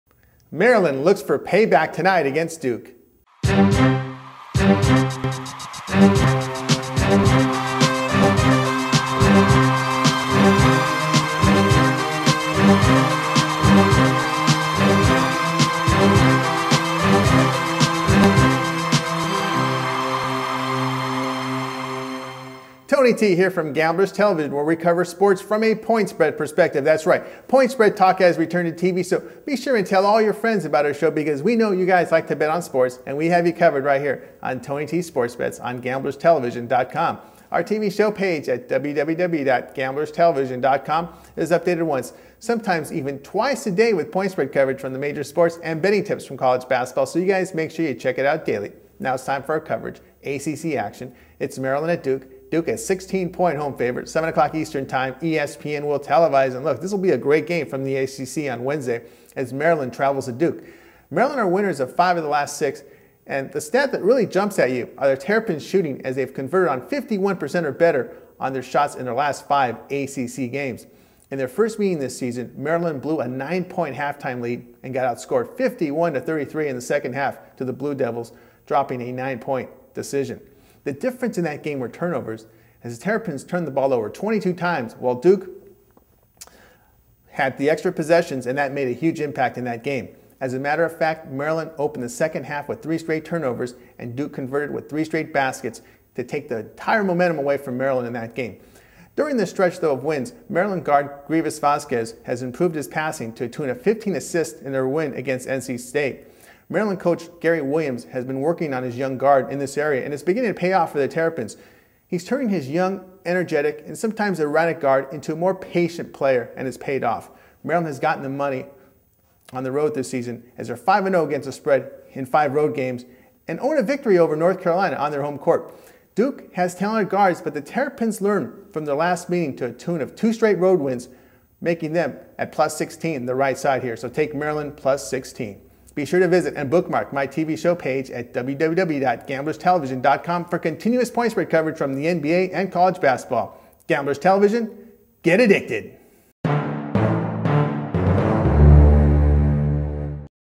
Maryland @ Duke College NCAA Basketball Preview
Tony T Breaks down the Big College Basketball TV Games and offers his selections on this edition of Tony T' Sports Bets airing now Game previewed is Maryland Terrapins @ Duke Blue Devils. for more videos like these.